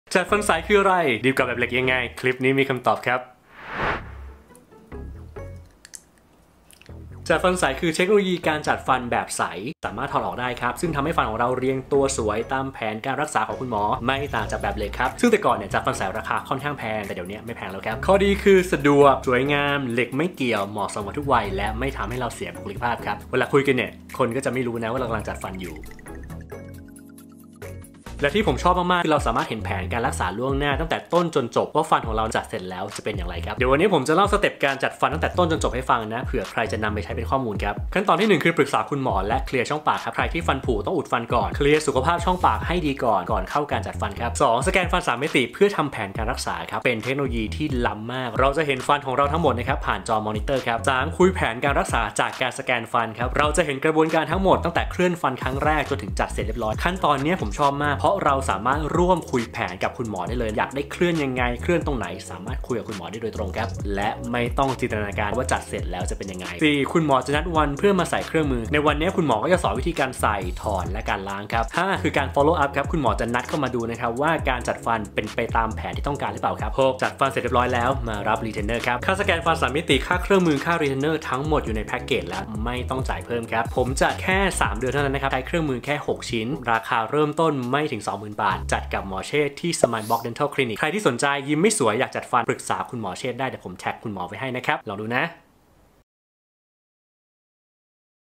The Magic Bank X MOOV #2
ครูแบงค์สอนหล่อ ขอเล่าต่อจากคลิปเมื่อวาน!
จัดฟันเหล็ก VS จัดฟันใส แตกต่างกันยังไงบ้าง?
คลิปนี้ครูแบงค์จะเล่า ขั้นตอนการจัดฟันใส ทั้งหมดให้ฟัง!
👑 จัดฟันใสราคาเริ่มต้นไม่ถึง 20,000 บาท! ถูกมากแต่ราคาคือคุ้มเกินคุณภาพ! MOOVClealAligners มีให้เลือกถึง 4 Package! เคสของครูแบงค์เลือกใช้ Package เครื่องมืออุปกรณ์เพียงแค่ 6 ชิ้นเท่านั้น! จัดฟันในระยะเวลาเพียงแค่ 3 เดือนเอง เพราะปัญหาฟันของครูแบงค์มีไม่เยอะมาก ...ใครอยากรู้ว่าจะต้องเลือก Package อะไร? ใช้เวลากี่เดือน? กี่ปี? ส่งภาพปัญหาฟันของคุณมาให้ SmileExpert ประเมินรอยยิ้มเบื้องต้นก่อนได้ค่ะ ✅

CR.Tik Tok @themagicbank